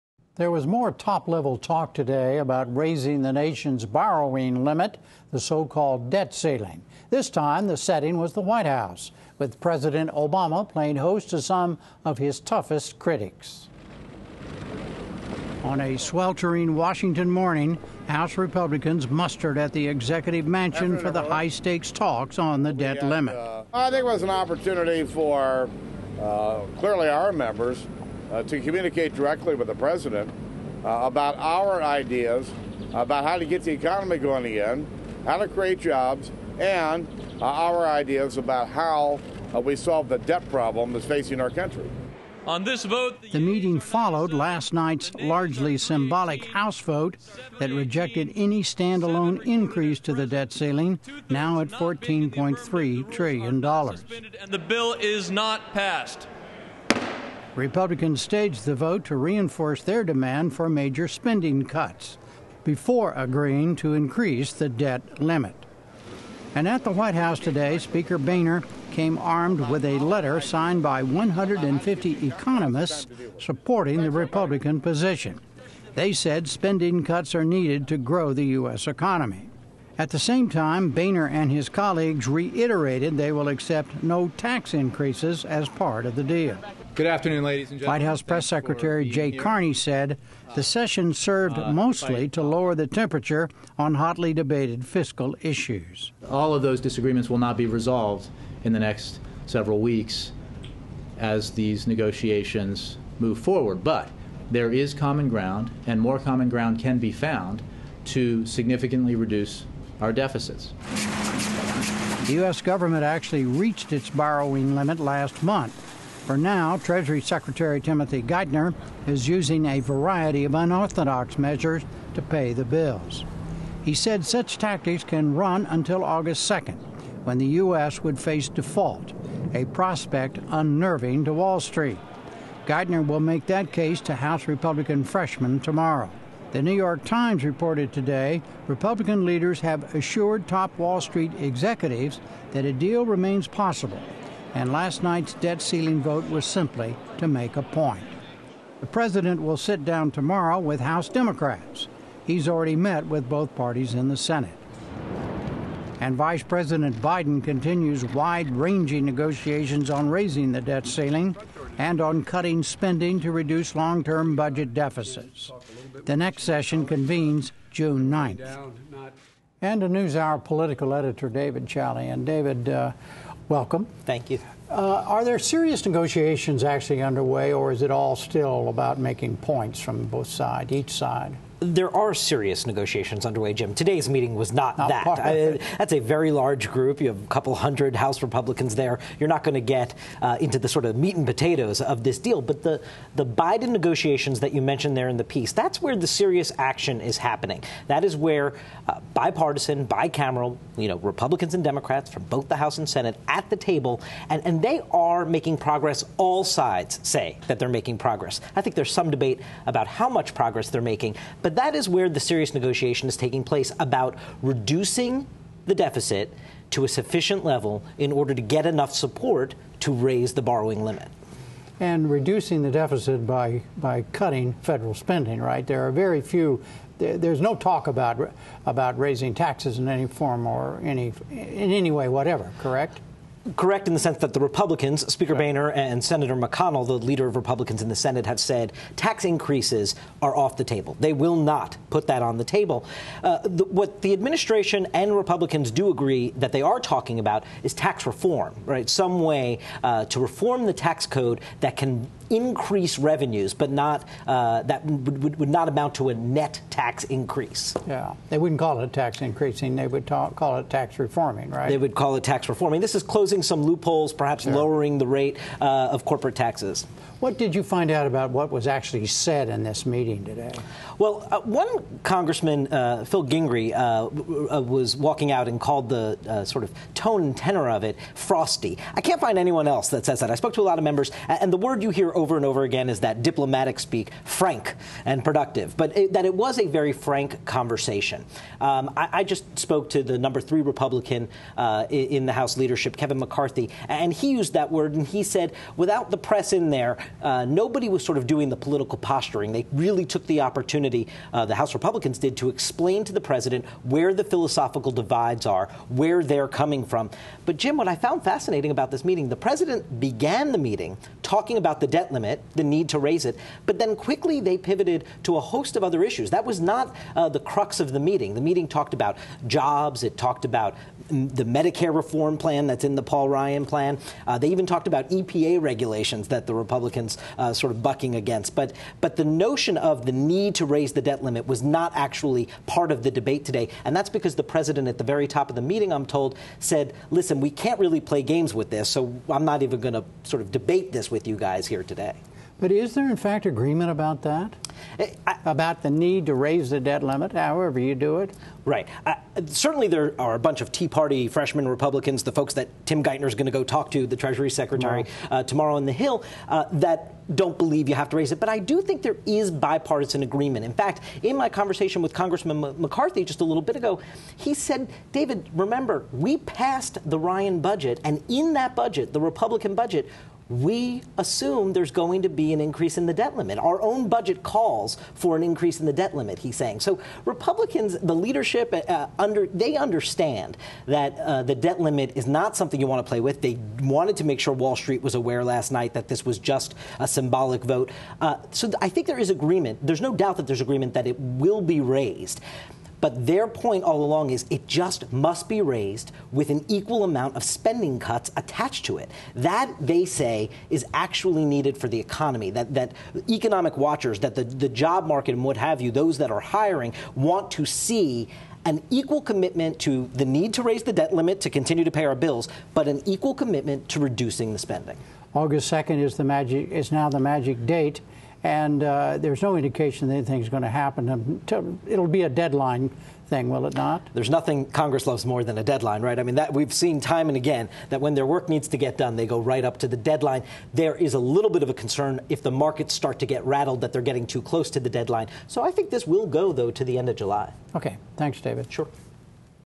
Amid 'Serious Negotiations' on Debt, Can Obama and GOP Find Common Ground?
President Obama met with congressional Republicans Wednesday to talk about a debt-reduction deal, but none was made. The meeting followed Tuesday night's largely symbolic House vote that rejected any stand-alone increase to the debt ceiling. Jim Lehrer discusses the prospects for a deal with Political Editor David Chalian.